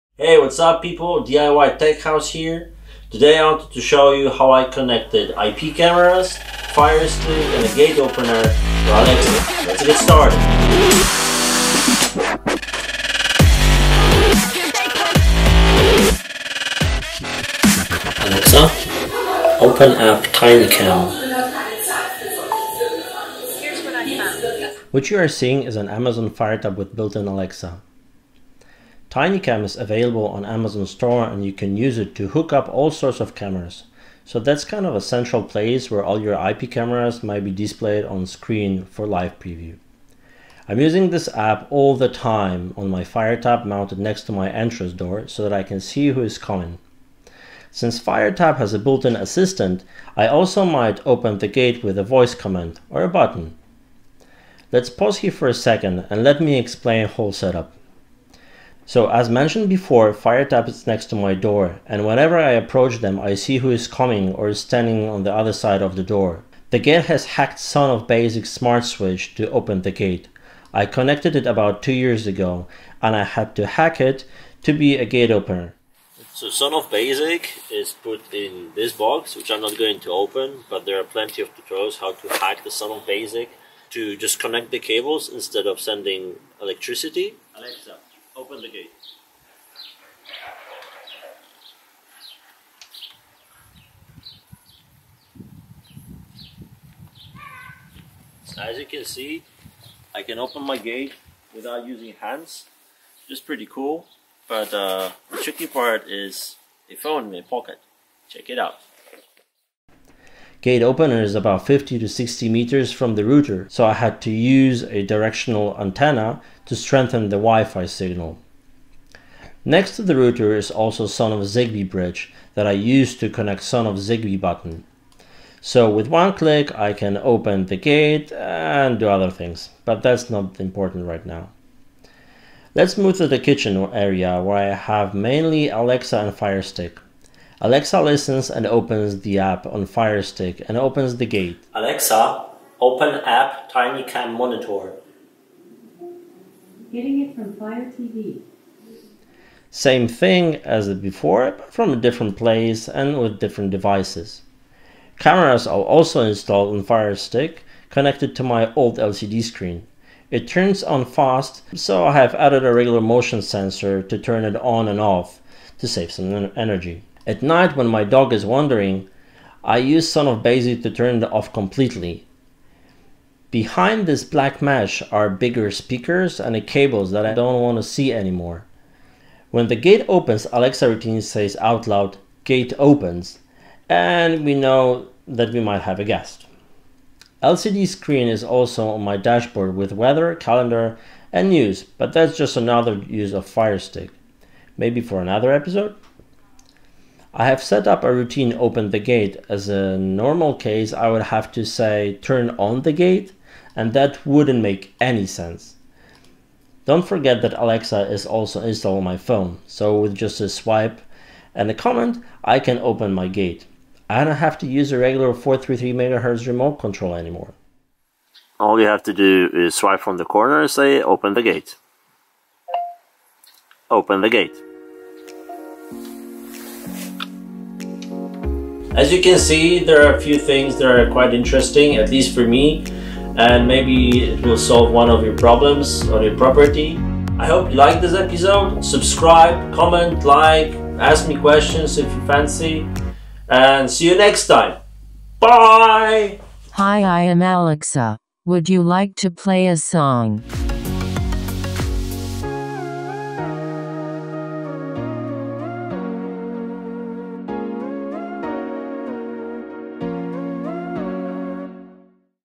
tinyCam IP monitoring through Alexa | Fire TV Stick | Fire HD Tablet
Home IP camera monitoring with Firestick, Alexa and Firestick. Sonoff switches are also mentioned in this video as I use it for opening the gate and using Firestick.